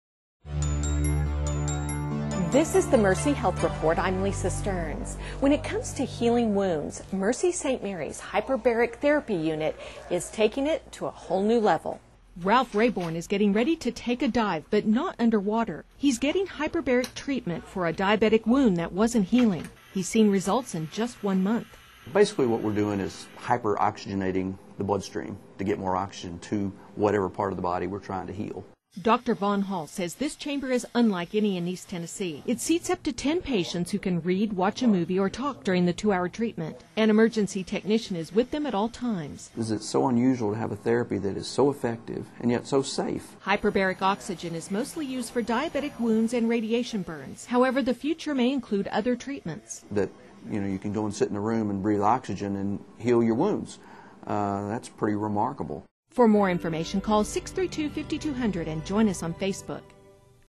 Mercy Health Report - July 2011 - Hyperbaric Oxygen Therapy
When it comes to healing wounds, Mercy St. Mary's Hyperbaric Therpay Unit is taking it to a whole new level.

Ralph Rayborn is getting ready to take a dive, but not underwater.  He's getting treatment for a diabetic wound that wasn't healing.  He's seen results in just one month.

"Basically what we're doing is hyperoxygenating the bloodstream to get more oxygen to whatever part of the body we're trying to heal," said Vaughan Hall, MD, medical director of the HBOT program at Mercy St. Mary's.

Dr. Hall says Mercy's unit is unlike any in East Tennessee. It seats up to 10 patients who can read, watch a movie or talk during the two-hour treatment.  An emergency technician is with them at all times.

"This is so unusual to have a therapy that is so effective and yet so safe," said Dr. Hall.

Hyperbaric oxygen is used mostly for diabetic wounds and radiation burns, however the future may include other treatments.

"It's pretty remarkable that you can go sit in a room, breathe oxygen and heal your wounds," said Dr. Hall.